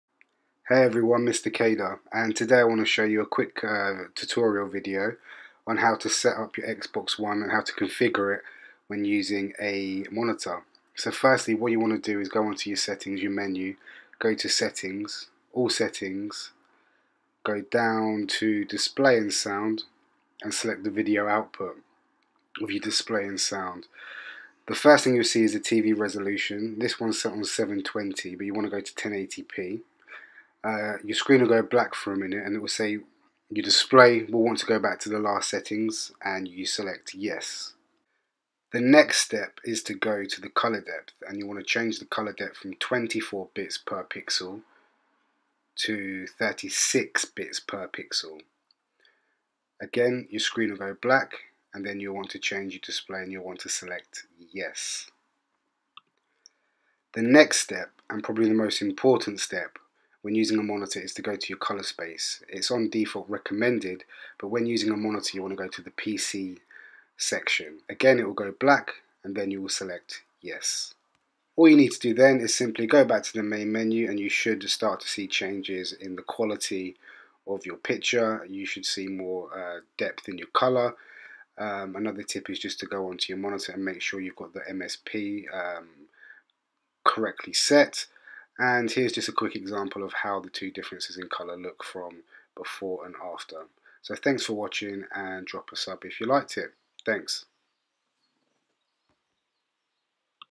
How to set up your xbox one with a gaming monitor
A quick tutorial on how to configure your settings on an xbox one with a gaming monitor.